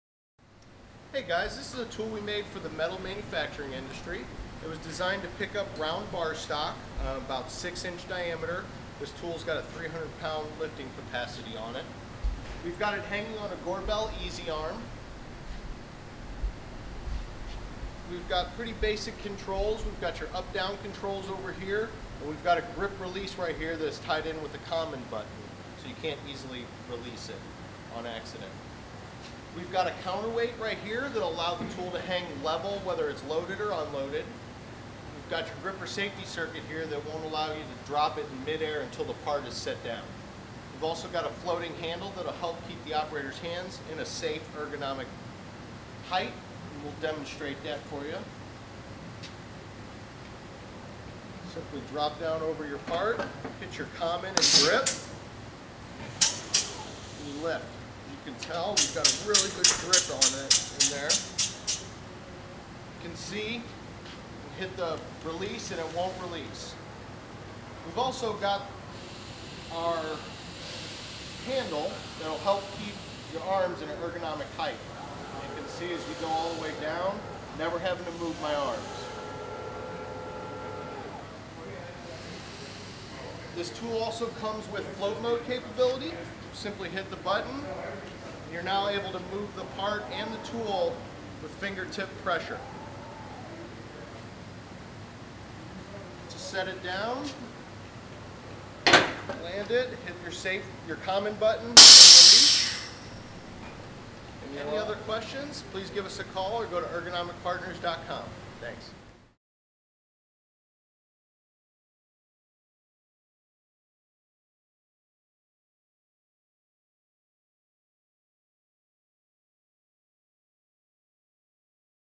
Bar Stock Gripper Tool
This custom lifting device grips and lifts bar stock. The gripper features an aluminum and steal powder coated frame, ergonomic floating handle, Linatex rubber covered grip jaws, float mode, momentary proportional up/down controls and grip/release controls with a common button. The tool is integrated with a Gorbel Easy Arm and equipped with our anti-drop circuit preventing operators from accidentally releasing the stock in mid-air.